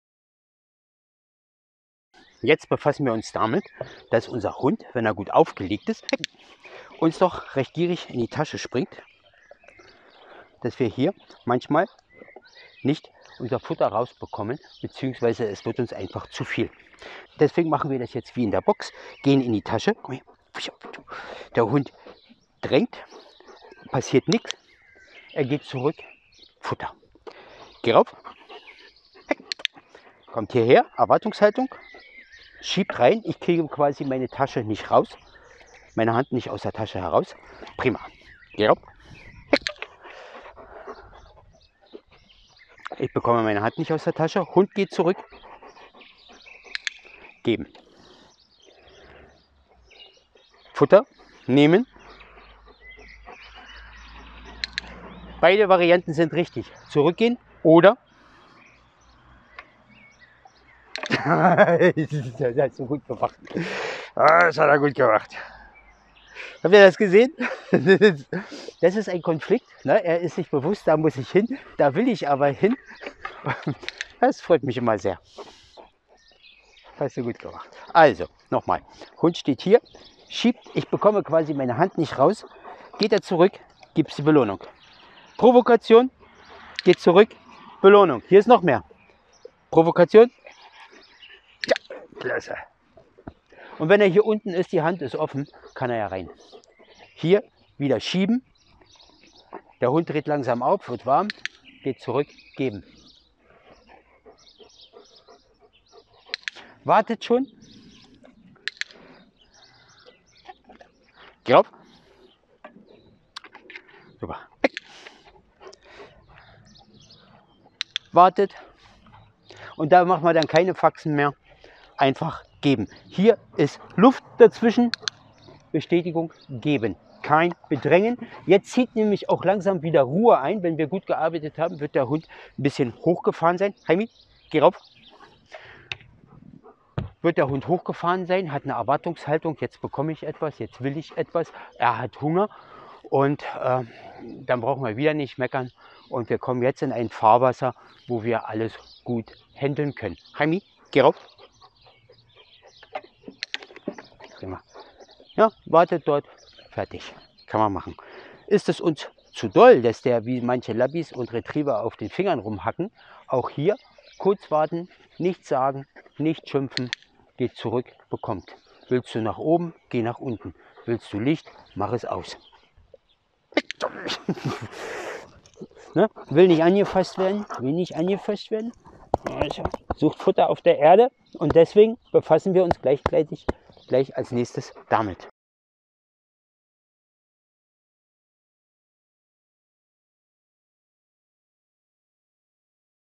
Wenn der Hund zu gierig ist
Aufgeregte und gierige Hunde können einem manchmal auf sehr unangenehme Art das Futter aus der Hand reißen. So lösen wir das Problem.